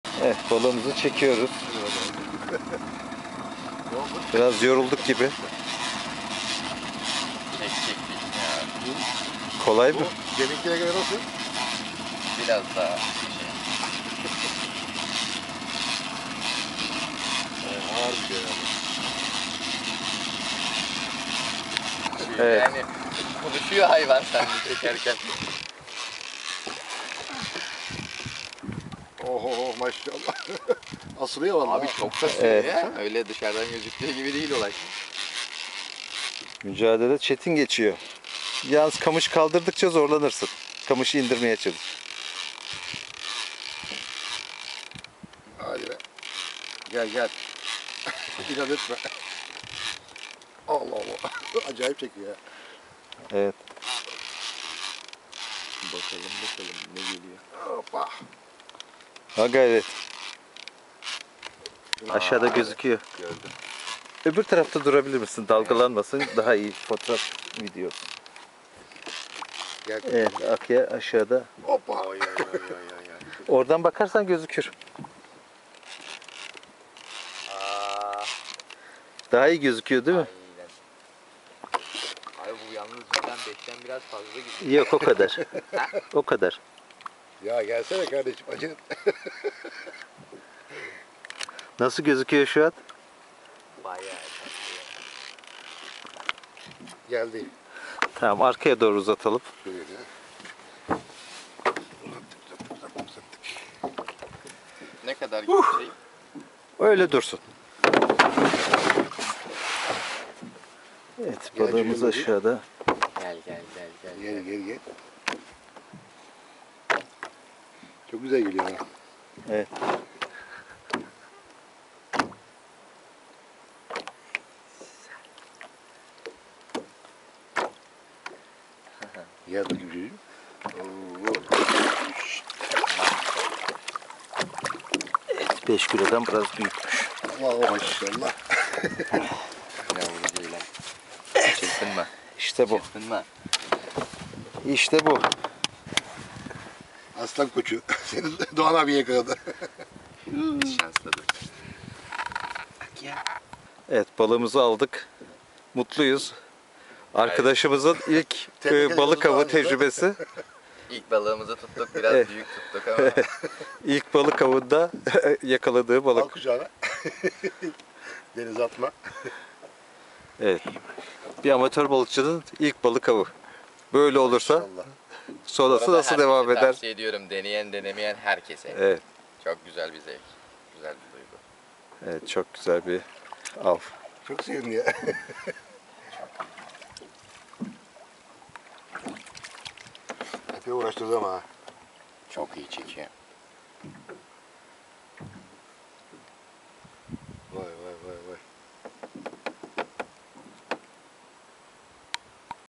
AMATOR BALIKCININ BALIK AVI SIFTAHI BOYLE OLDU
- Bir amatör balıkçı ilk defa katıldığı büyük balık avı böyle bir av yaparsa sonrası nasıl olur? Kısmetinin artarak devam etmesi dileğiyle.